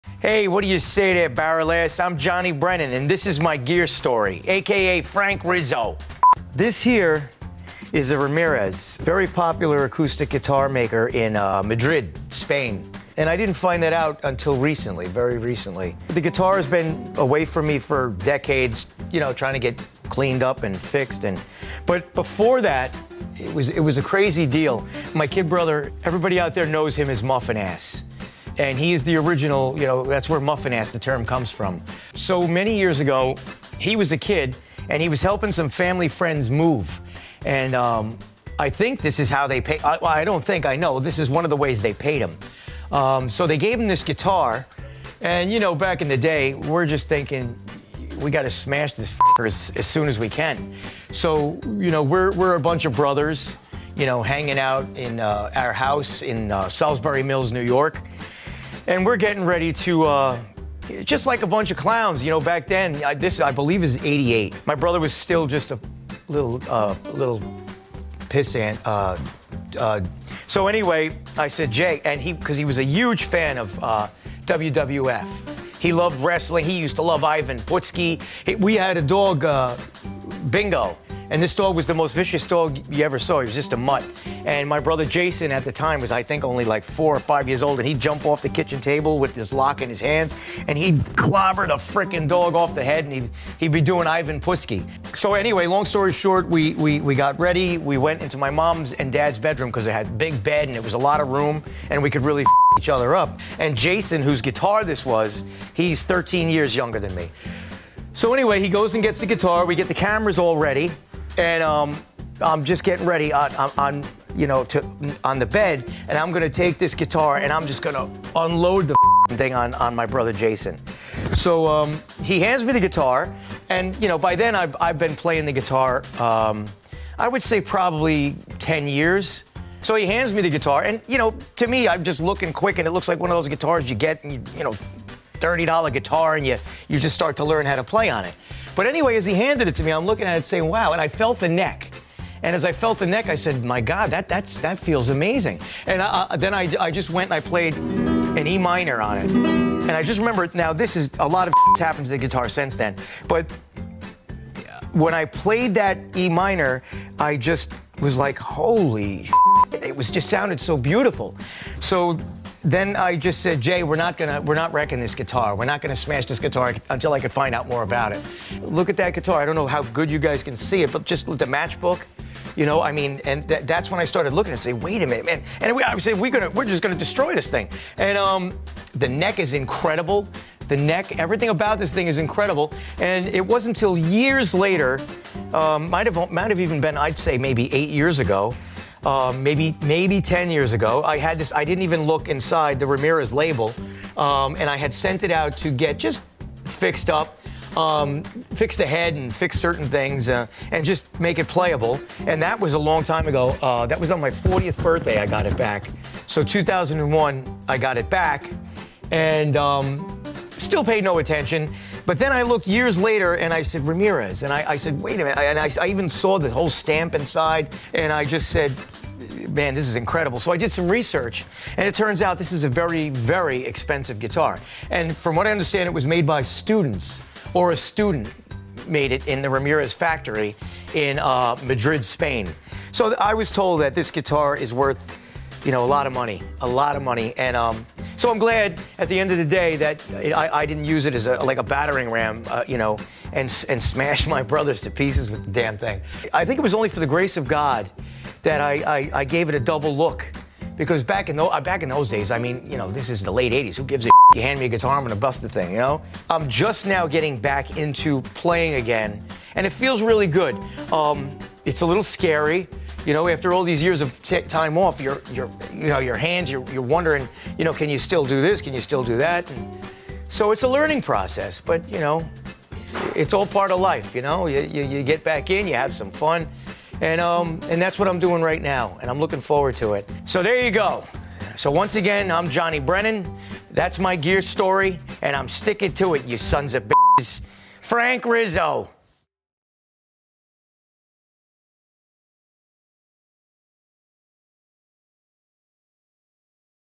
AMGearStories: Johnny Brennan's Ramirez Acoustic Guitar
You know him as the founder of The Jerky Boys and for voicing Mort Goldman on Family Guy. So when our friend Johnny Brennan offered to stop by and talk about his rare Ramirez acoustic guitar we jumped at the opportunity. From wrestling to student guitar builders in Spain, this AMGearStories episode covers it all!

⌚️ Timestamps⌚️
0:00 INTRO
0:16 WHERE'D IT COME FROM?
0:45 "LET'S SMASH THIS GUITAR"
1:49 LIGHTS, CAMERA, WRESTLING?
2:10 HOLD ON A MINUTE...
3:35 RAMIREZ GUITARS HISTORY
4:16 PICKING IT BACK UP
4:41 FRANK RIZZO OUTRO